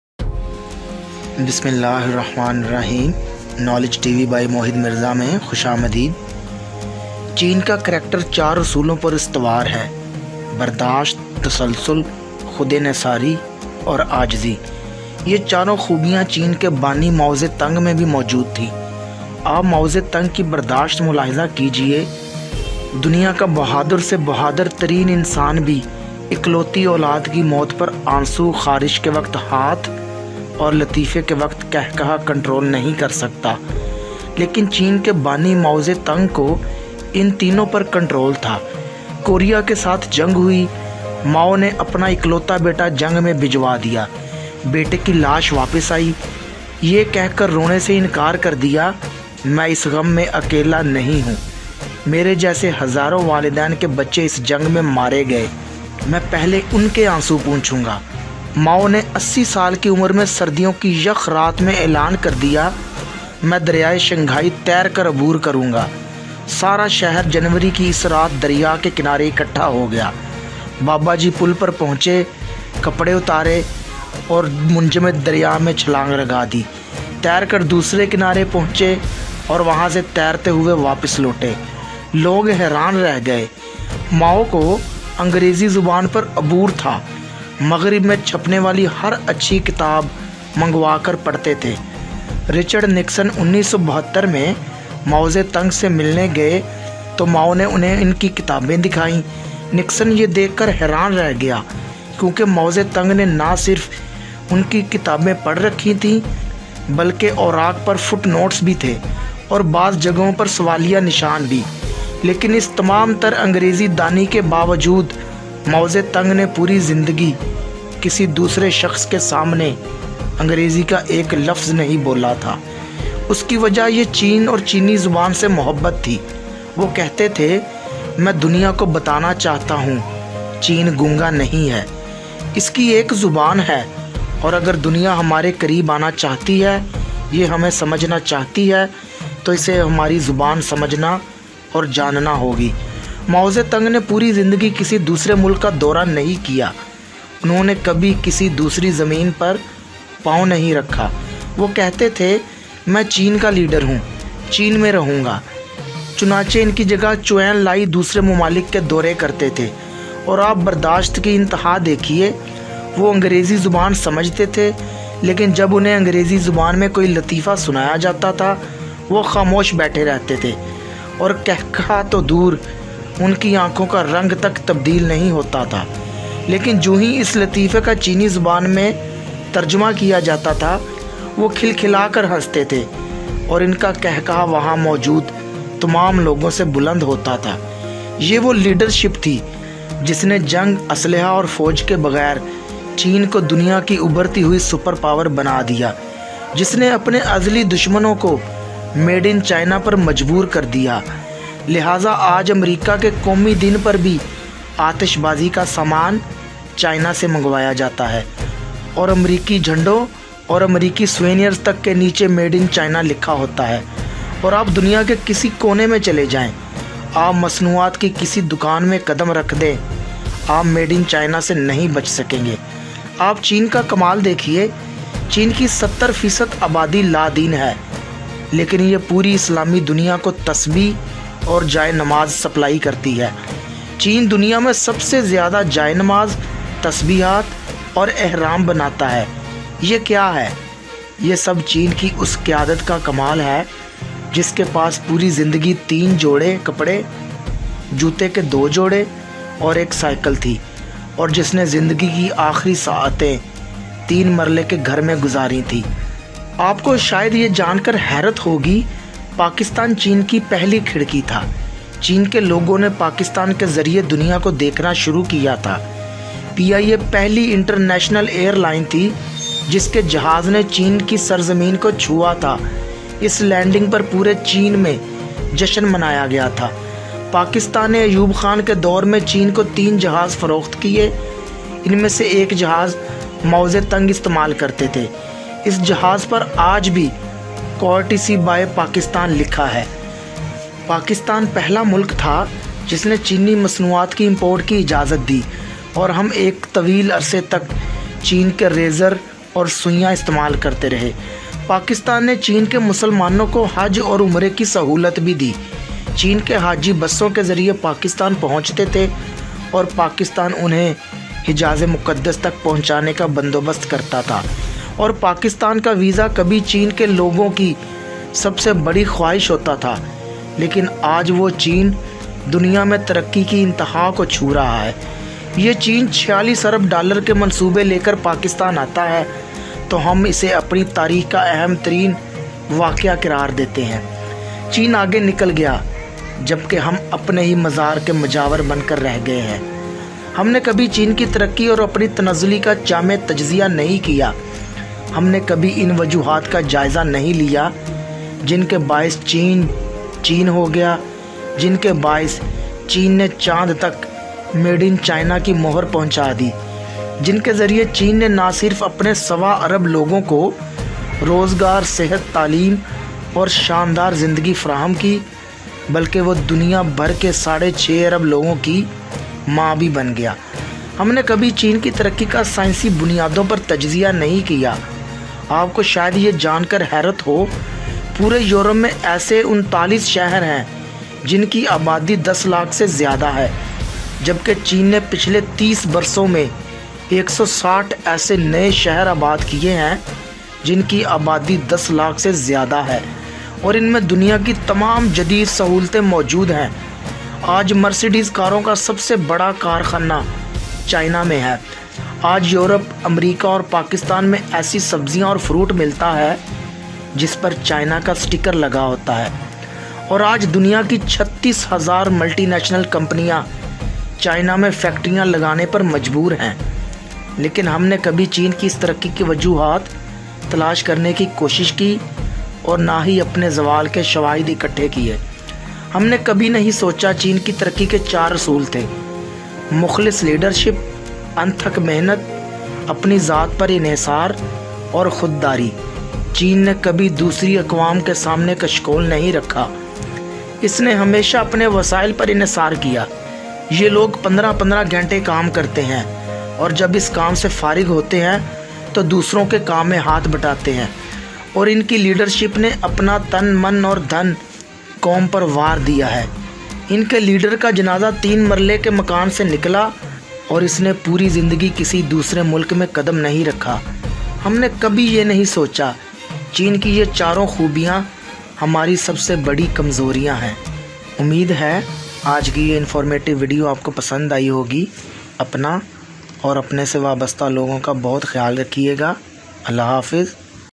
HOW TO DEVELOP CHINA | URDU | HINDI
BASED ON A TRUE EFFECTS.CHINA KI TARAQQI KE BARE MAI ENTRUSTING INFORMATION